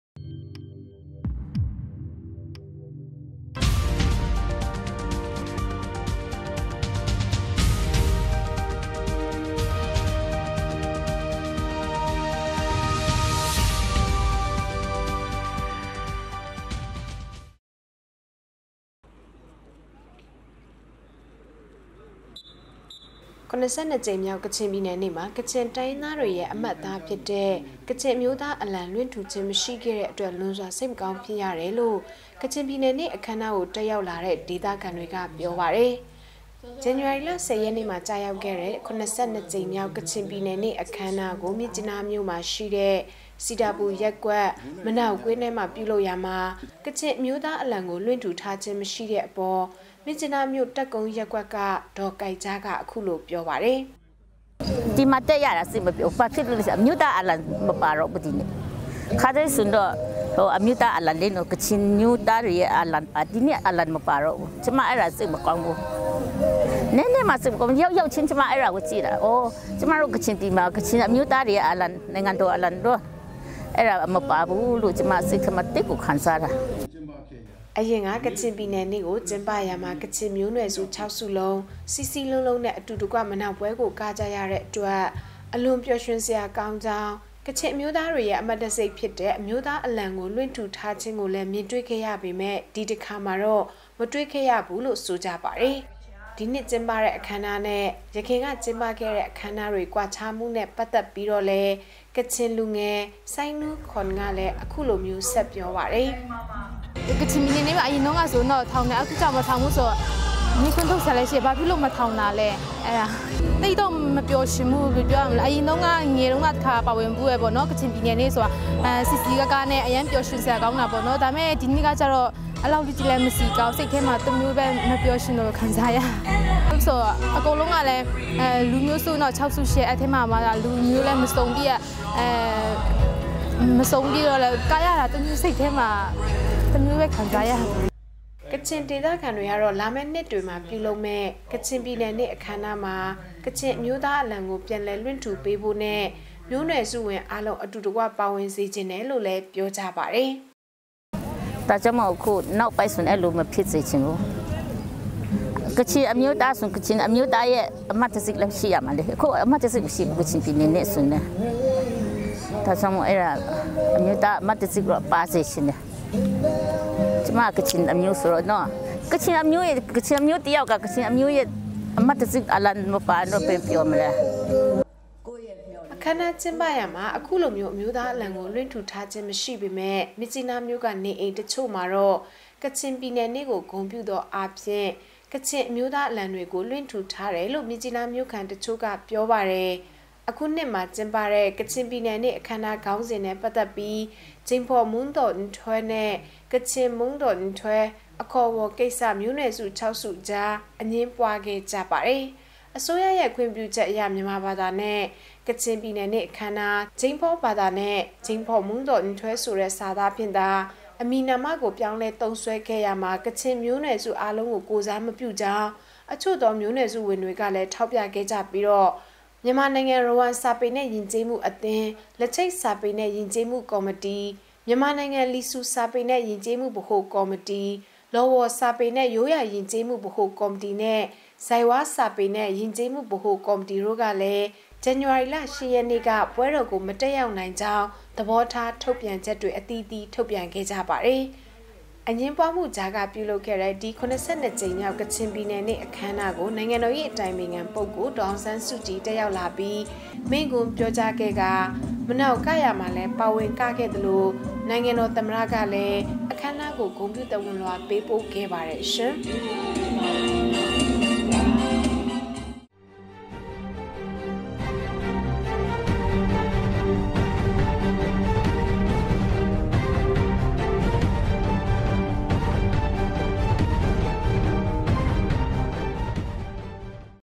ကချင်အမျိုးသား အလံမလွှင့်ထူတဲ့ ကချင်ပြည်နယ်နေ့
၇၂ ကြိမ် မြောက် ကချင်ပြည်နယ်နေ့တွင် ကချင်တိုင်းရင်းသားများ၏ အမှတ်အသားဖြစ်တဲ့ ကချင်အမျိုးသား အလံ လွှင်ထူခြင်းမရှိခဲ့တဲ့အတွက် လွန်စွာ စိတ်မကောင်း ဖြစ်ရတယ်လို့ ကချင်ပြည်နယ်နေ့ အခမ်းအနားသို့ တက်ရောက်လာတဲ့ ကချင်ဒေသခံတွေက ပြောပါတယ်။